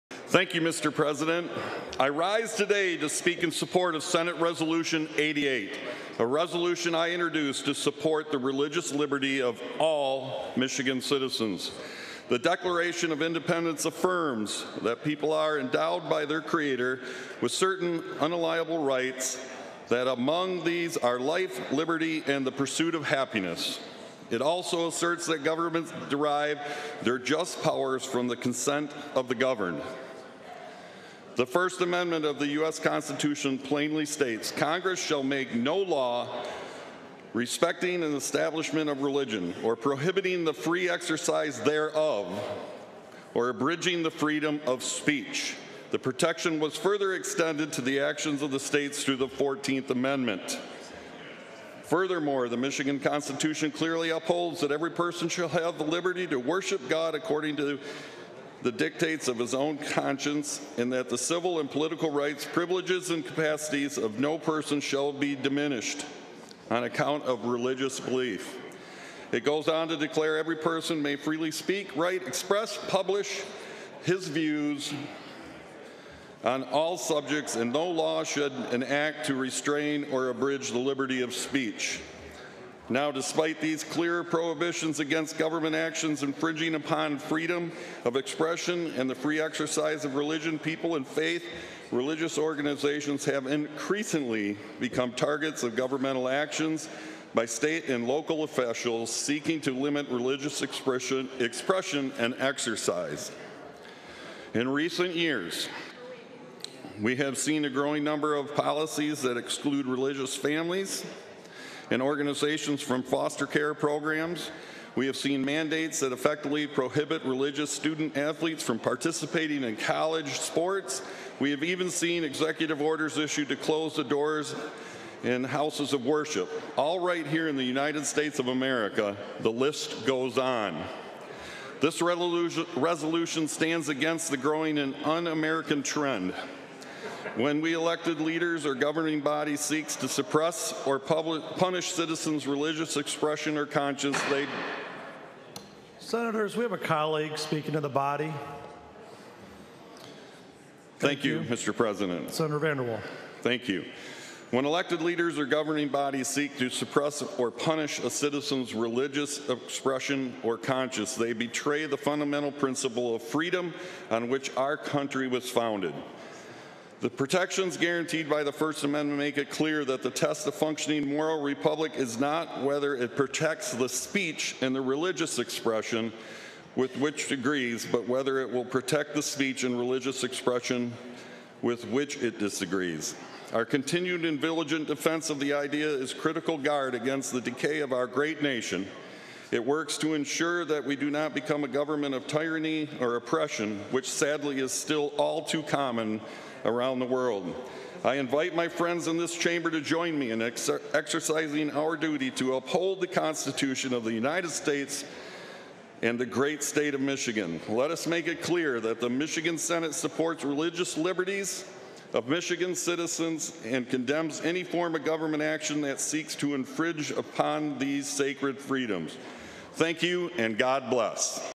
Senate adopts VanderWall resolution defending religious liberty
Senator Curt VanderWall speaks in support of Senate Resolution 88 to voice support for the religious liberty of Michigan citizens during Michigan Senate session on October 21, 2021.